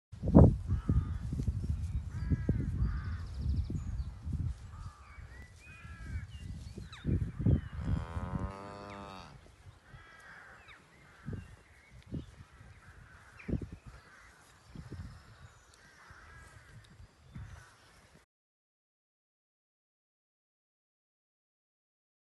70 Jersey/NZ Fr/Fr Calves
70 High EBI Calves for Sale.
All Homebred animals coming from a spring-calving grassbased system, delivering over 530kg/ms. Majority of the group are AI Sired January-March born.
 This group has great genetics that would be a great option for any new entrants.
 Contact Liam for more information.